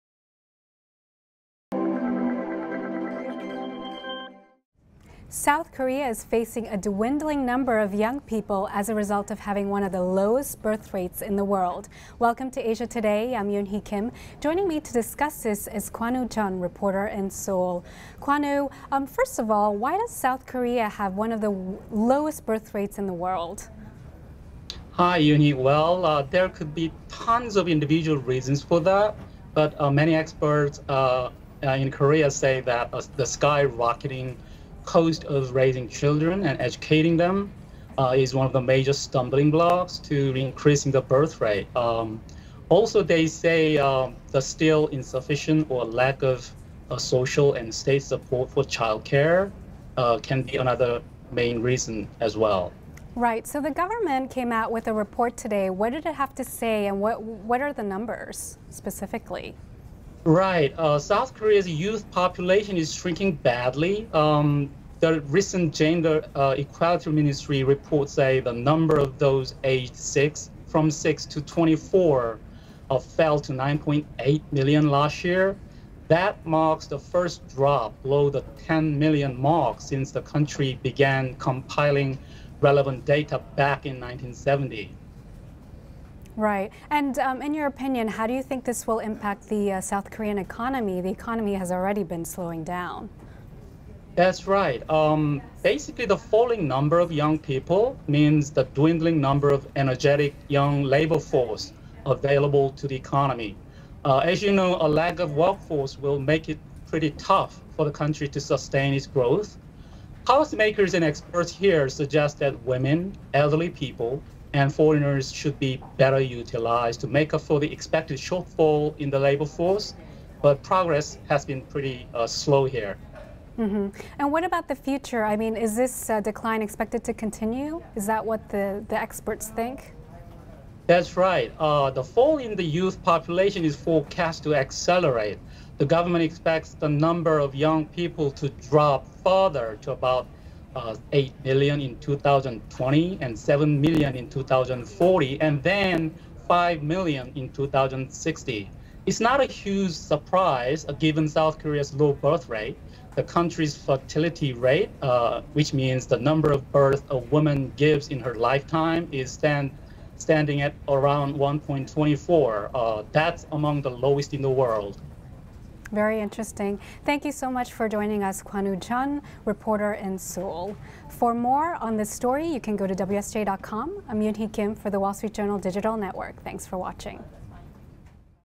Low Birthrate Shrinks South Korea's Youth Population
Due to a low birthrate, there are now fewer than 10 million young people in South Korea for first time in decades, a trend with implications for the country's economic future. The WSJ's Yun-Hee Kim speaks to reporter Kwanwoo Jun in Seoul.